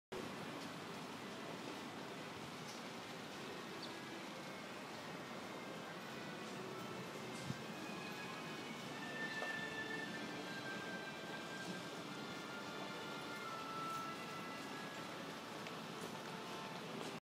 NISSAN New Mobility CONCEPT 公道走行
横浜元町を走ります。日本で初めての公道走行です。